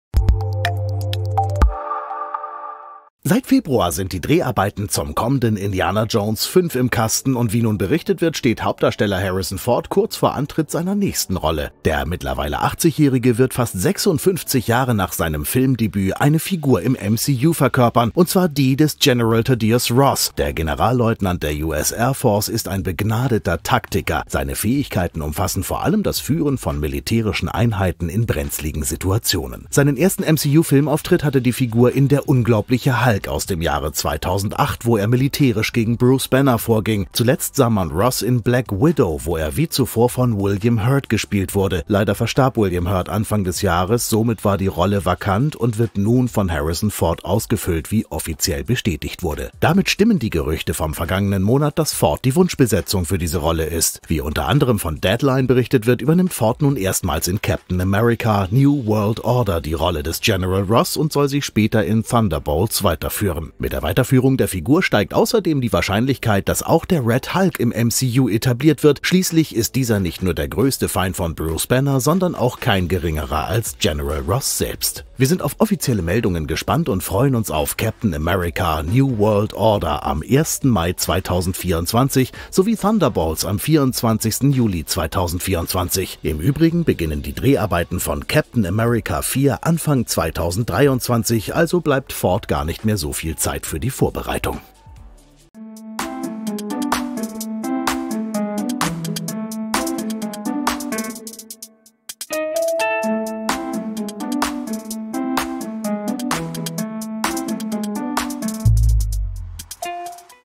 Wechselt Indie das Franchise?! - CAPTAIN AMERICA: New World Order (2024) - KinoCheck News
Eine Fortsetzung von Marvel Studios' The Falcon and the Winter Soldier und folgt Sam Wilson als dem neuen Captain America.

Note | CaptainAmericaNewWorldOrder German KinoCheckNews courtesy of Walt Disney Company. Bei einem Kauf ändert sich für euch nichts, aber ihr unterstützt dadurch unsere Arbeit. | KinoCheck®